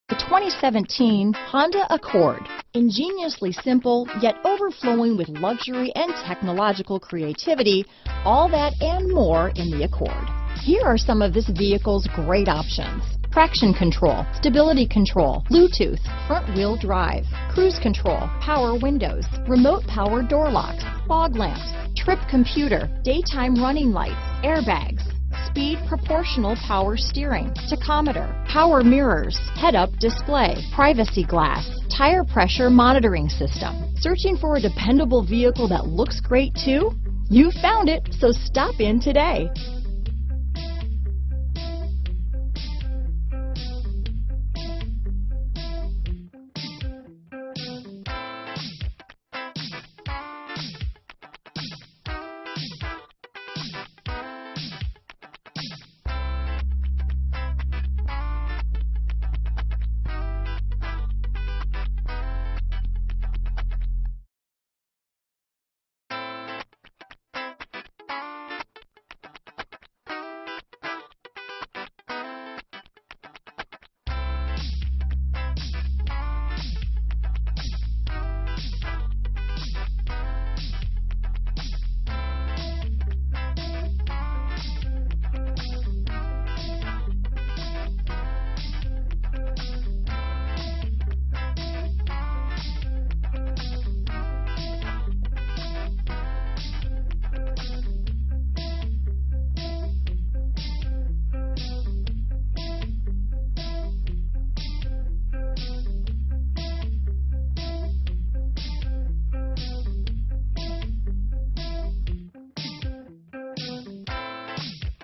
2017 Honda Accord Sedan Homestead, Miami, Kendall, Hialeah, South Dade, FL 57079
2017 Honda Accord Sedan
2017 Honda Accord Sedan Sport - Stock#: 57079 - VIN#: 1HGCR2F53HA191566

Largo Honda
554 NE 1st Ave

SAVE AT THE PUMP!!! 34 MPG Hwy... Right car! Right price!! Just Arrived. It does everything so well, except be lazy! Safety equipment includes: ABS, Traction control, Curtain airbags, Passenger Airbag, Front fog/driving lights...Other features include: Bluetooth, Power locks, Power windows, CVT Transmission, Climate control... Largo Honda Home Delivery for Less!